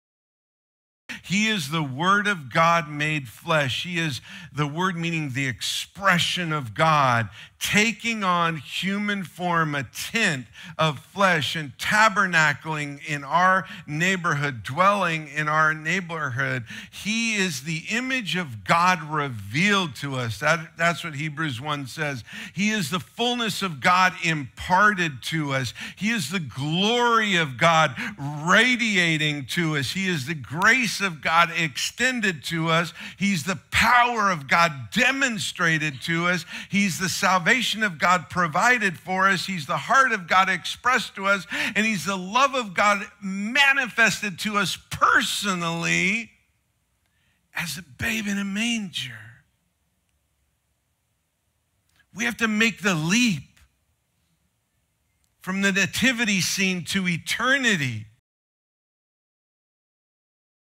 Who Jesus Is
Jesus Christ is the power of salvation born in a manger to die on the cross, for you, personally.